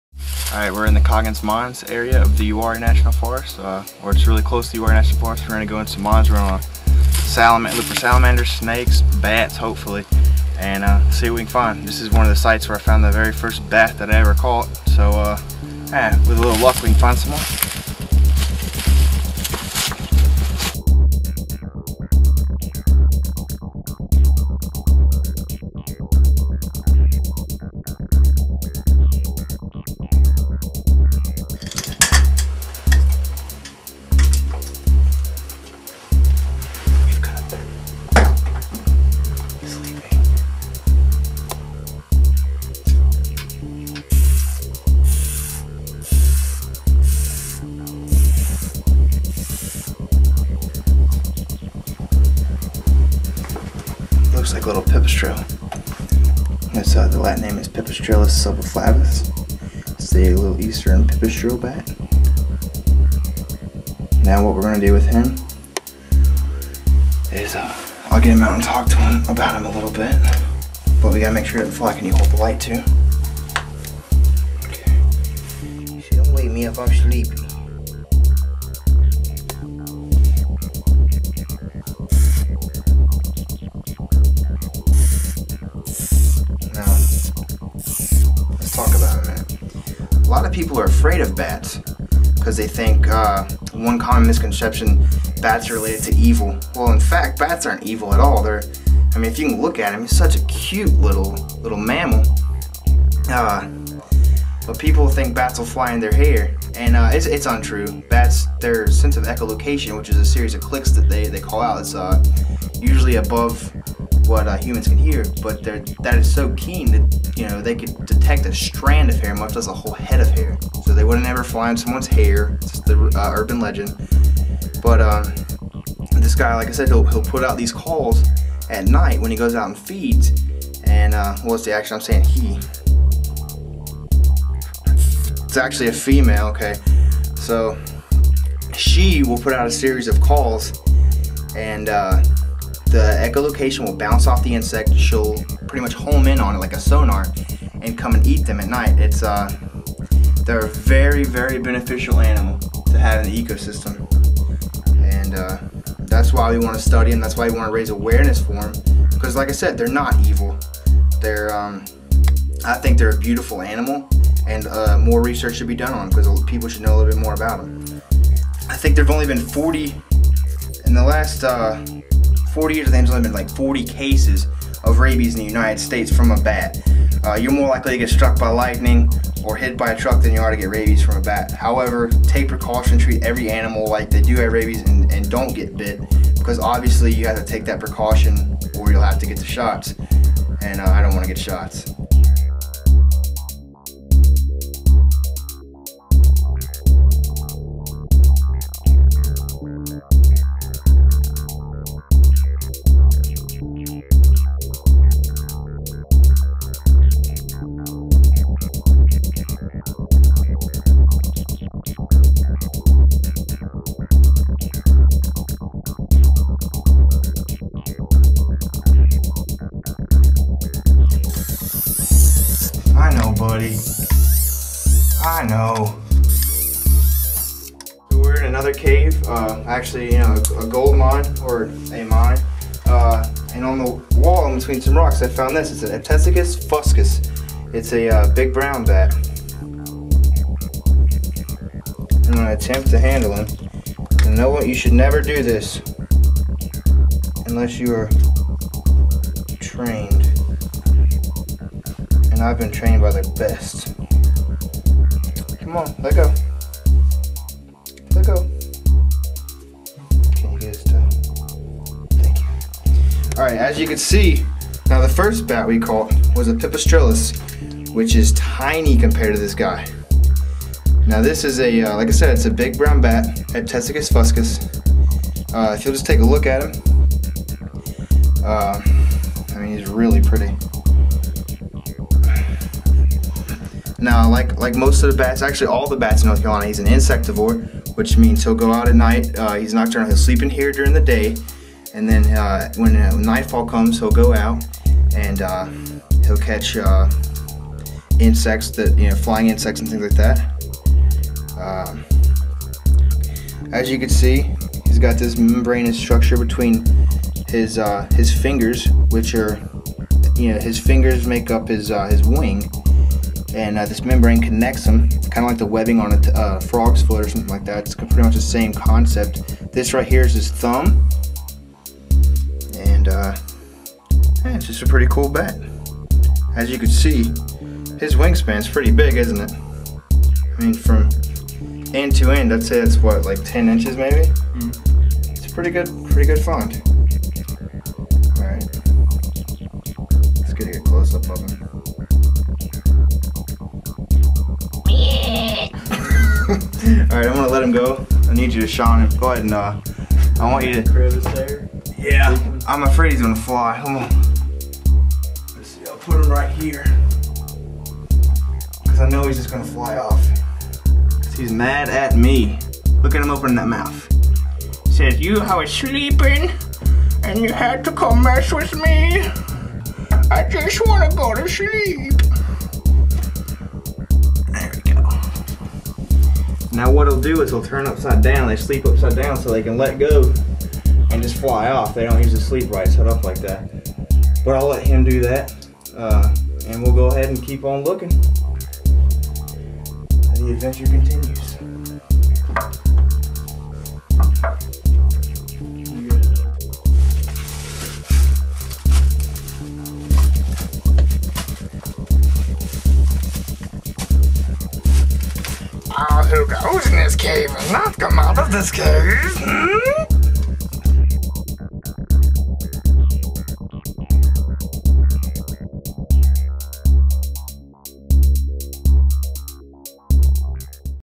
Adventure Archive--Bats in Coggins Mine 2006
This Adventure Archive takes us back to 2006 to the Coggin's Mines near El Dorado NC.  Many of these mines are now closed off and it is encouraged that you don't attempt to handle bats.  I worked in a research lab that studied bats when I was an undergraduate at UNCG and gained experience in proper handling and identification.  Many species of Bats in the US are now suffering from a condition called White Nose Syndrome and it has been suggested that human traffic in mines and caves could potentially spread this fungal infection to yet unreached areas.  Never handle bats unless you are properly trained.  These animals are very valuable to ecosystems and fit a specific niche.  They are truly wonderful animals!